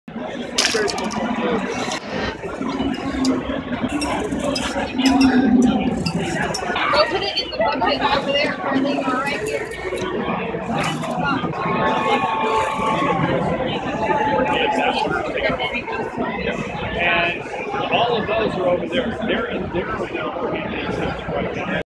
First Robotics: Girl almost successfully puts a block in a basket
On April 21, the First Robotics Competition (FRC) took place in Houston, TX. Outside of the competition, you could look at vendors. One of the vendors had an Edubot and kids could learn how to get the robot to pick up a block and put it in the basket.